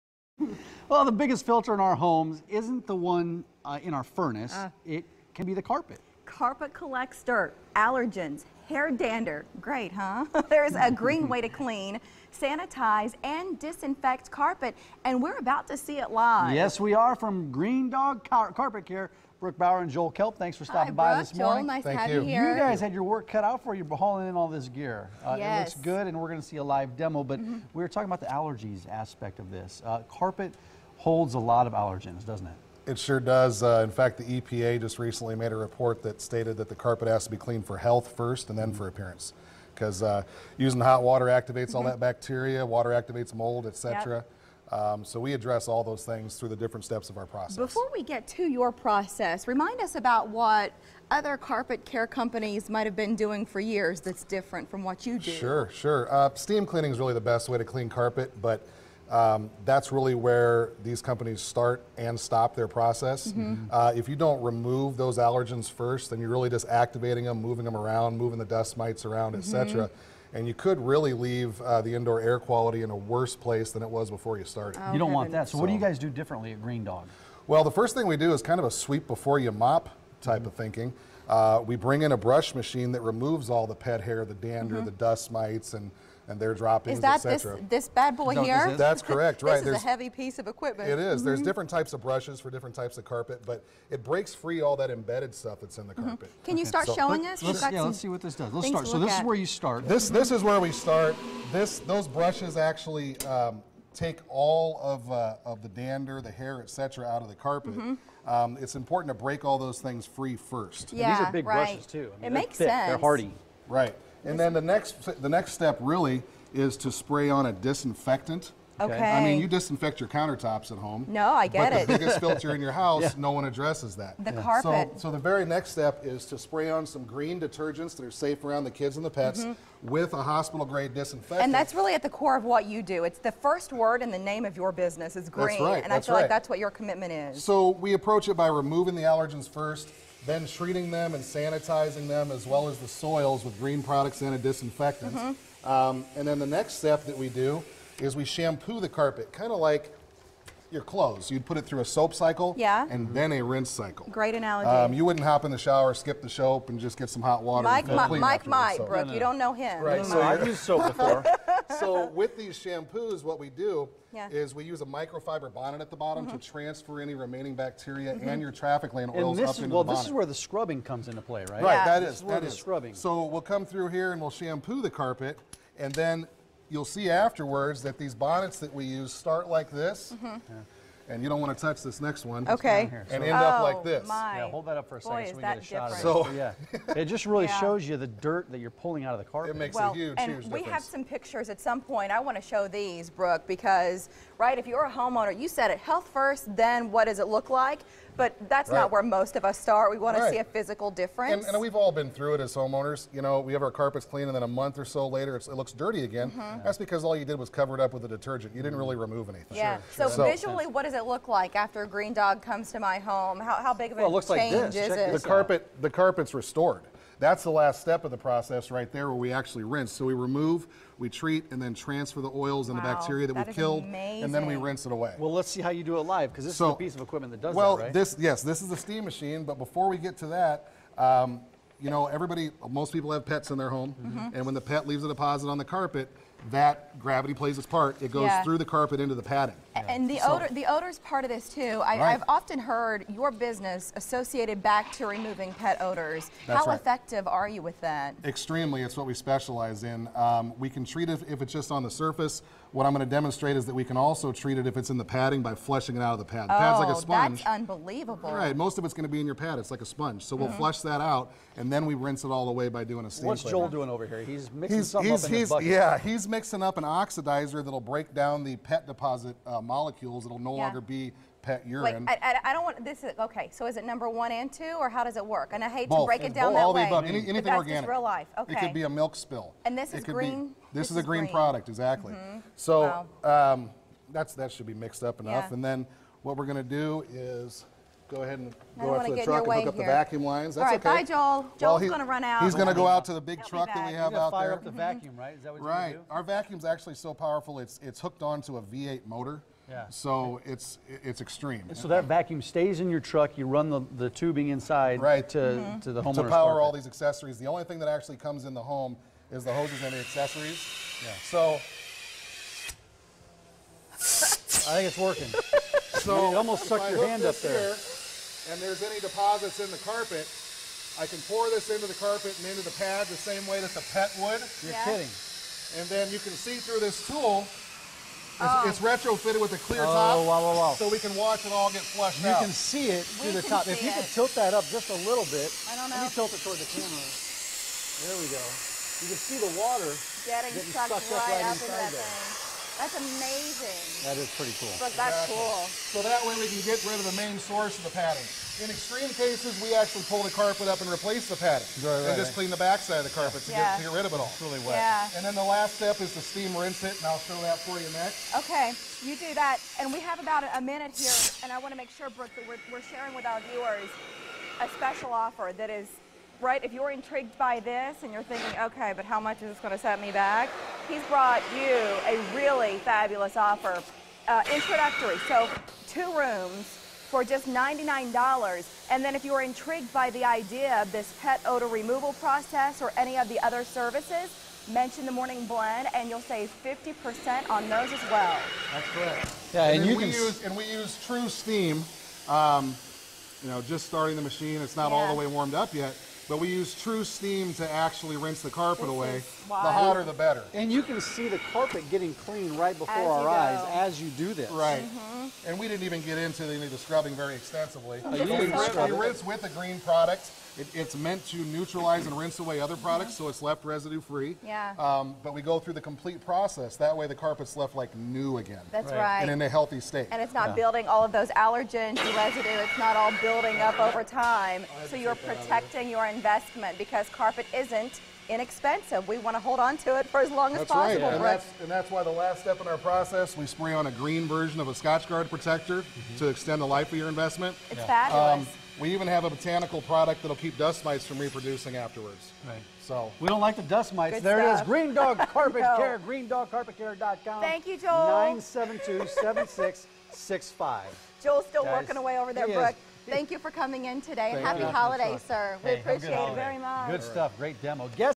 GreenDog Carpet Care
The US Environmental Protection Agency reports that carpet is the biggest filter in the home, collecting millions of mites, allergens and bacteria.

Green Dog Carpet Care can fix this issue with their cleaning process that kills and removes allergens and oils first, followed by a comprehensive Steam Cleaning to rinse the carpet reside free!